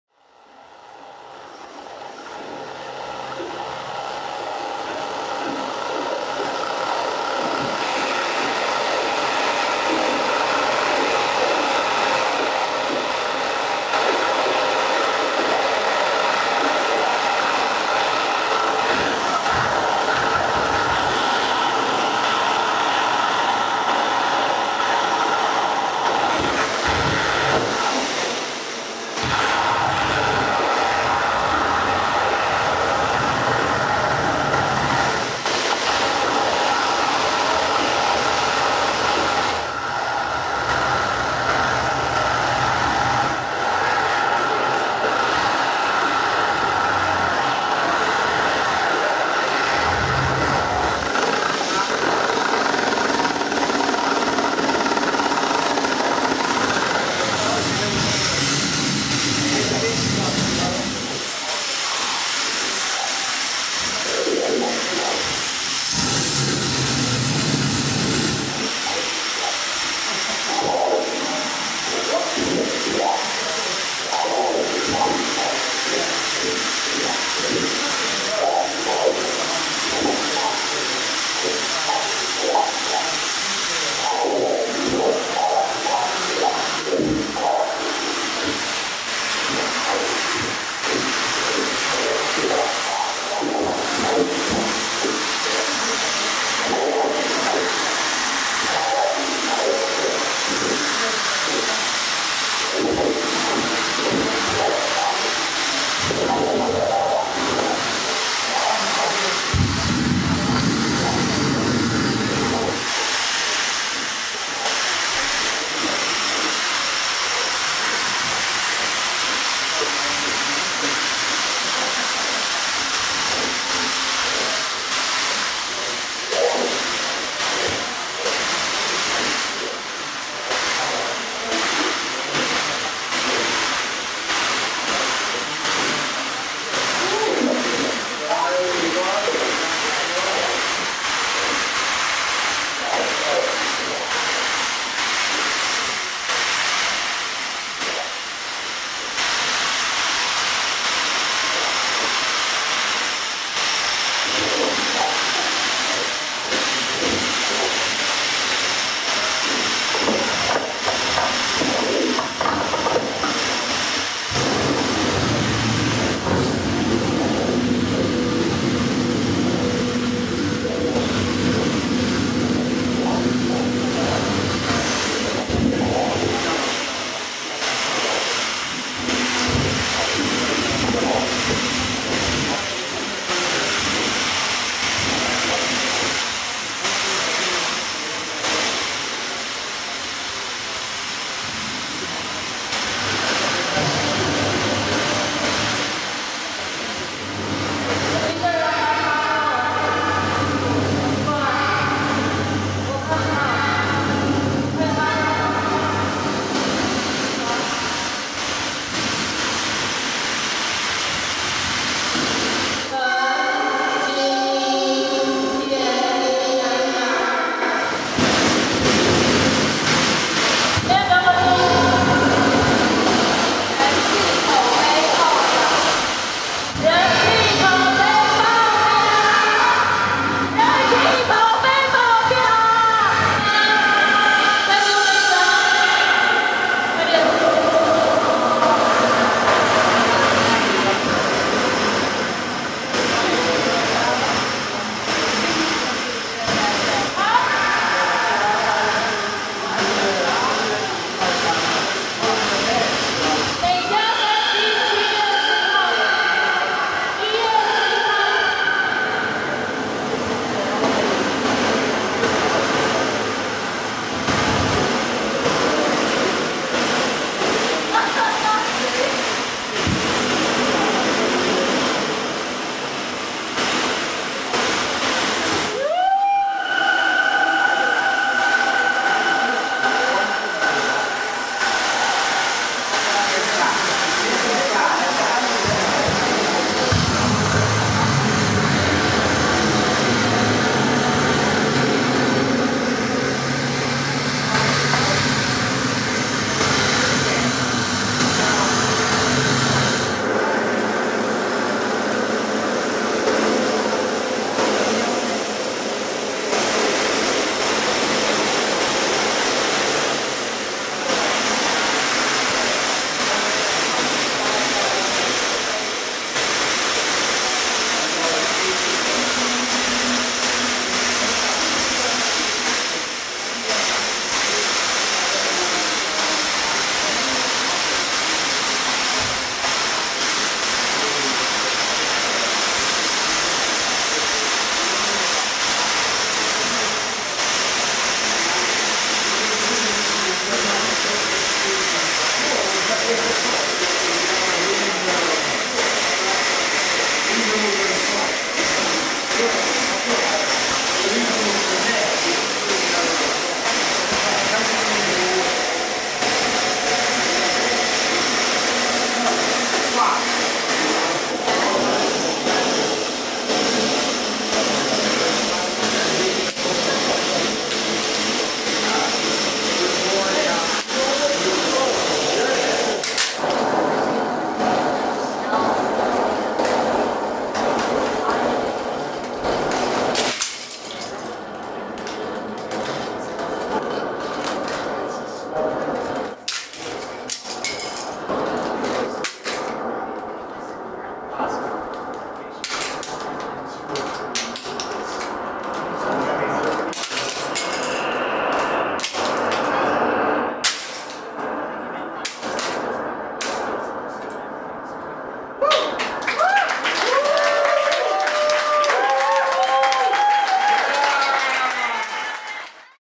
Betty Apple at DEPOPULATE 01 Presented by White Fungus
Depopulate 01 is the first in a series of events by White Fungus in Taiwan. Depopulate 01 features performances by EVOL (Barcelona), Samin Son (South Korea / New Zealand), Wang Fujui (Taipei), and Betty Apple (Taipei).  Video by Damien Owen Trainor.